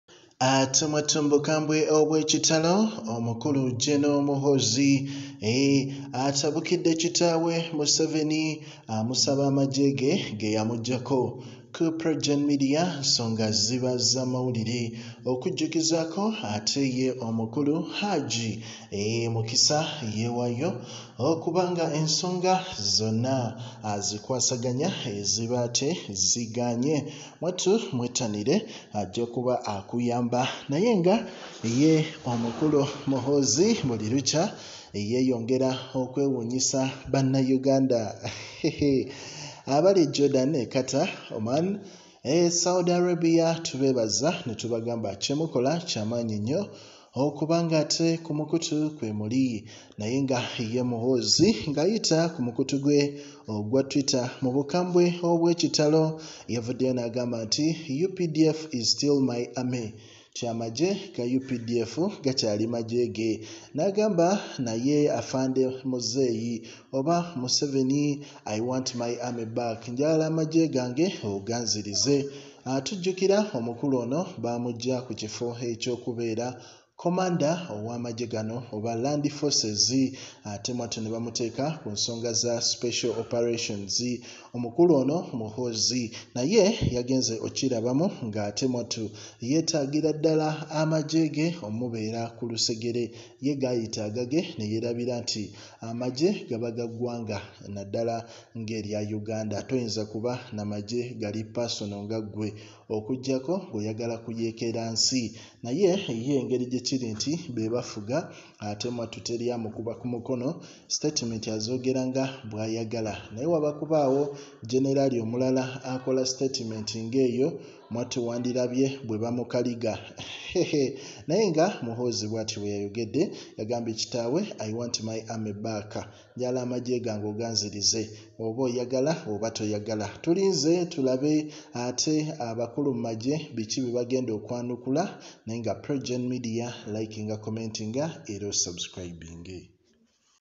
M7 Gamubugumye Ekiri mu State House Muhoozi Atabukidde kitaawe museveni Wuuno amusabye amudize amaje
I Want My Army Back – Gen Muhoozi Kainerugaba Sends Strong Warning To Father
Controversial senior presidential advisor, Gen Muhoozi Kainerugaba, has sounded out a strong warning to his father, President Yoweri Kaguta Museveni.
According to Muhoozi, the UPDF is his army. He, therefore, wants it back. Muhoozi made sure to put the ‘my army’ in quotation marks to drive his point home
UPDF is still ‘My’ Army. Afande Mzee, I want my army back,” tweeted Muhoozi.

It should be noted that Muhoozi is the commander of the Land Forces for about two years. He was however removed from this role last year and promoted to the rank of a General from Lieutenant-General.

This was after several controversial tweets about security issues in the region. This however didn’t help to arrest the situation as he’s continued to air out these controversial topics via his Twitter page.